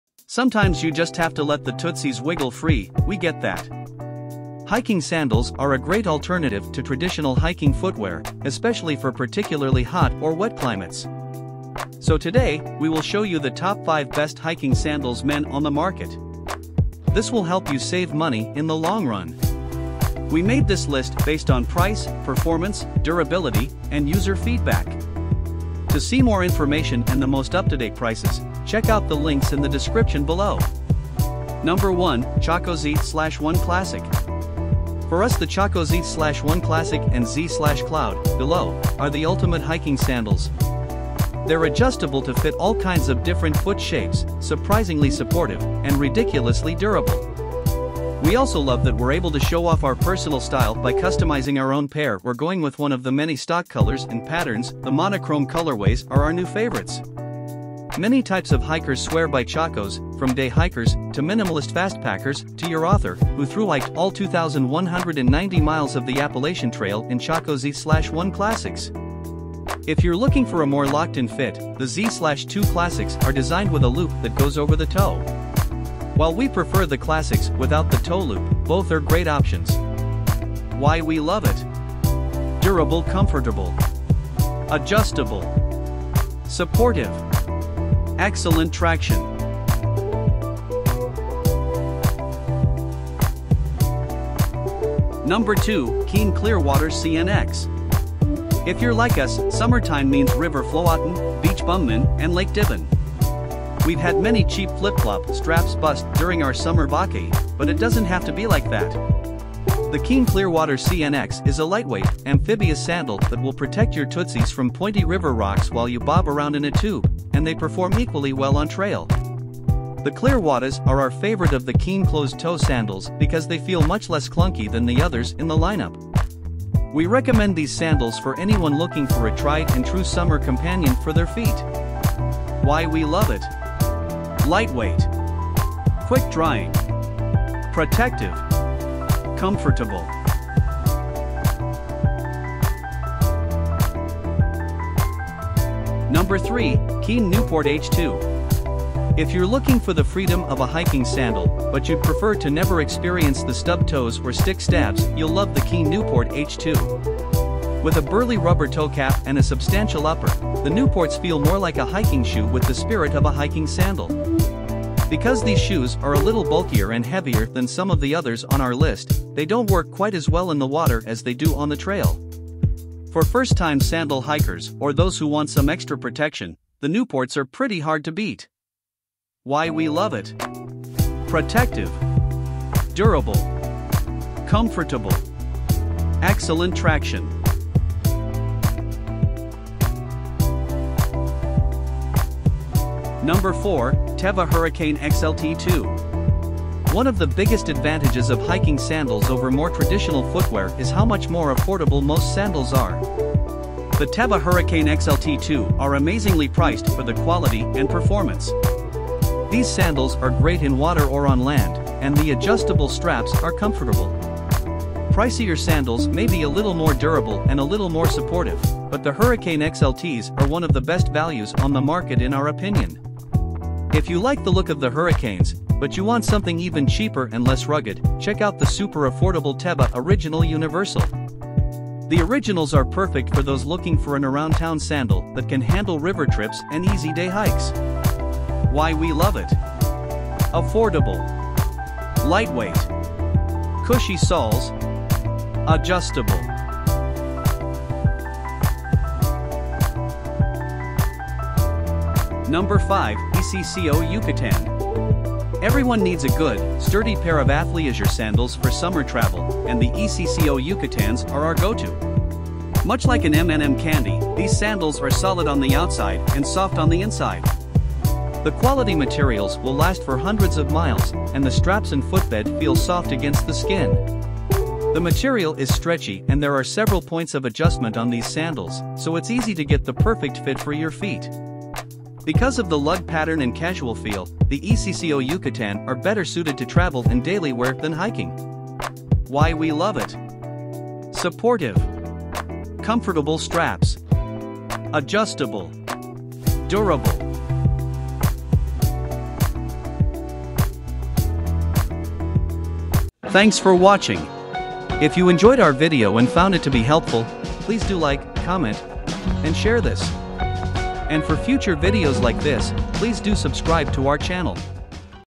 Top 5 Best Hiking Sandals for Men (Reviews 2024)
Chaco Z/1 Classic
KEEN Clearwater CNX
KEEN Newport H2
Teva Hurricane XLT2
ECCO Yucatan

Hiking sandals are a versatile and comfortable option for hikers of all levels. However, with so many different brands and styles on the market, it can be difficult to know which one is right for you. Here are a few things to keep in mind when choosing the right hiking sandal:

First, consider the climate you'll be hiking in. If you're planning on tackling a more challenging trail, you'll want a sandal that provides good traction and support. If you're only going on shorter hikes or well-maintained trails, you may get away with a lighter, more minimalist sandal.

Next, think about the type of closure you prefer. Some hiking sandals have laces that need to be tied, while others have Velcro straps or buckle closures. Consider how easy it will be to put your sandals on and take them off on the trail.

Finally, make sure to try on your hiking sandals before purchasing them. Everyone's feet are different, so finding a sandal that fits well and provides enough support is important. Once you've found the perfect pair of hiking sandals, you'll be ready to tackle any trail confidently and comfortably.

0:00 - Intro
0:34 - Chaco Z/1 Classic
1:36 - KEEN Clearwater CNX
2:26 - KEEN Newport H2
3:14 - Teva Hurricane XLT2
4:12 - ECCO Yucatan
5:06 - Outro

hiking sandals
sandals for hiking
sandals for backpacking
men's backpacking sandals
men's hiking sandals